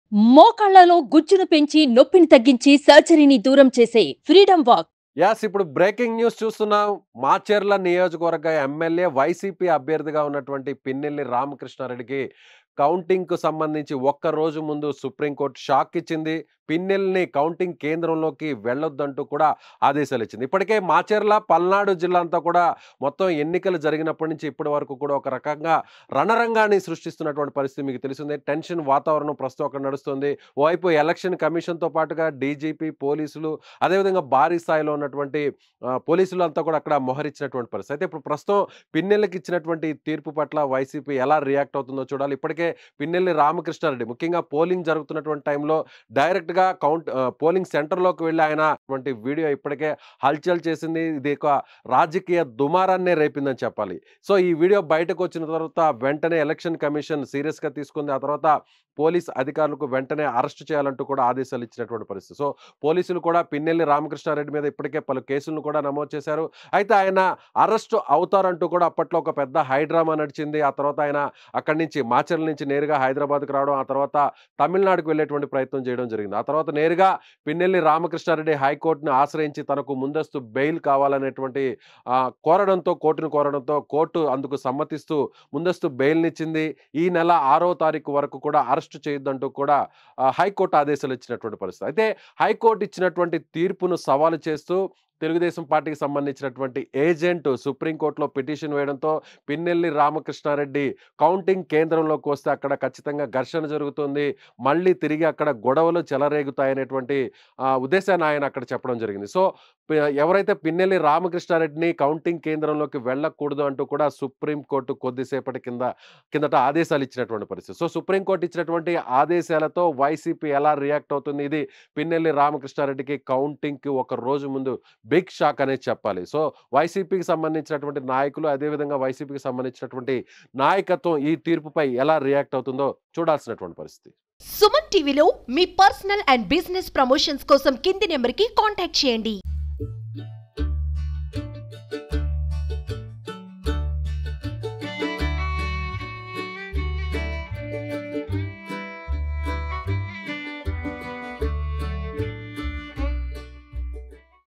పిన్నెల్లి కి సుప్రీం షాక్..కౌంటింగ్ కి వెళ్తే అరెస్ట్..? | Supreme Court Shock To Pinnelli
Watch►పిన్నెల్లి కి సుప్రీం షాక్..కౌంటింగ్ కి వెళ్తే అరెస్ట్..? | Supreme Court Shock To Pinnelli @SumanTVrampachodavaram